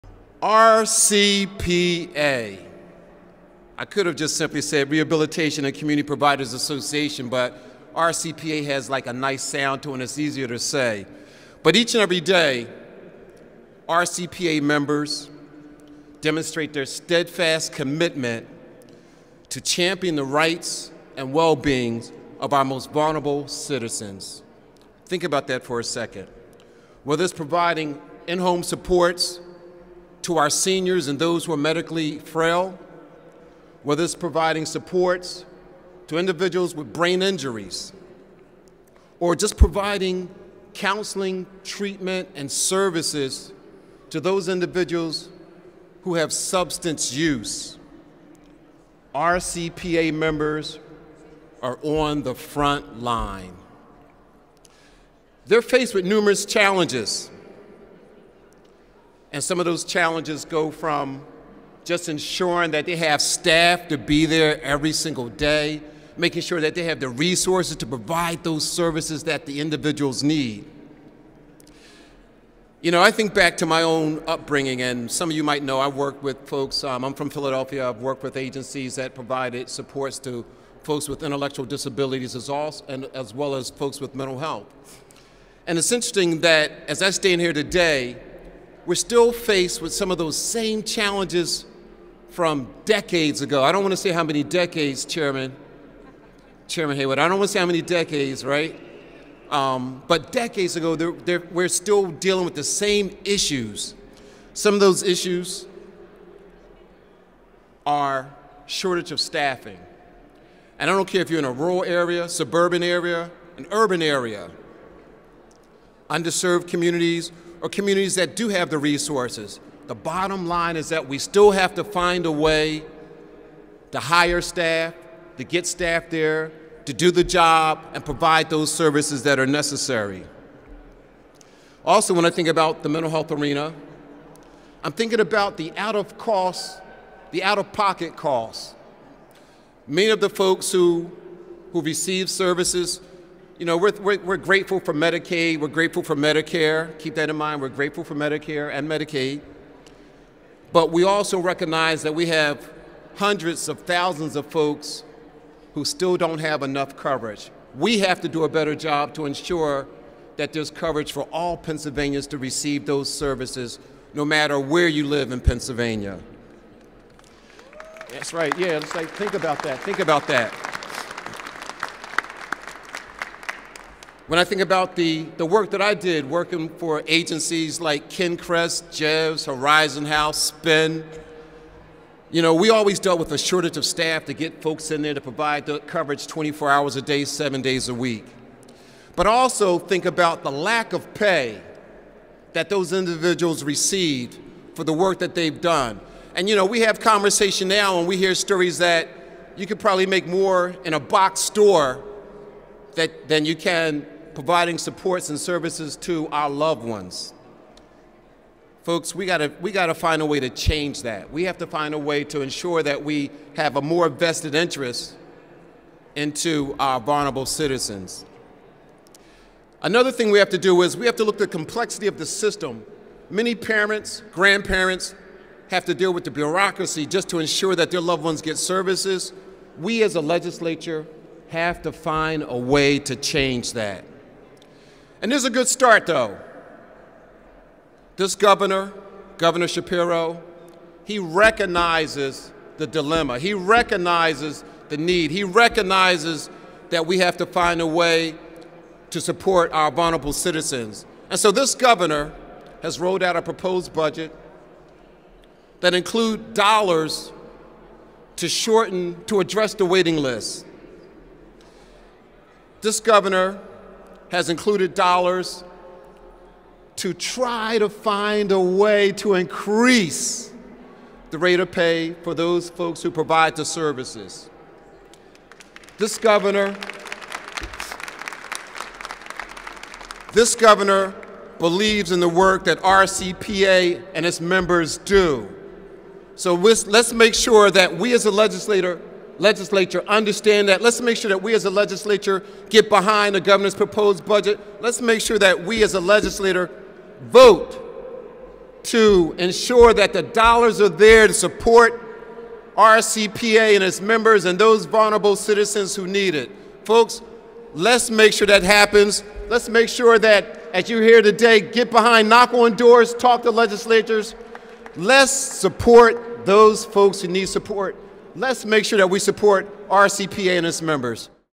Kinsey: RCPA Workers are on the Front Line
At a Capitol rally with the Rehabilitation and Community Providers Association, Pa. House Human Services Committee Chairman state Rep. Stephen Kinsey highlights the challenges facing direct care providers, including low wages and a lack of staffing. Kinsey says elected officials must find a way to deliver the resources needed so urban, suburban and rural communities can receive the life-sustaining care they need.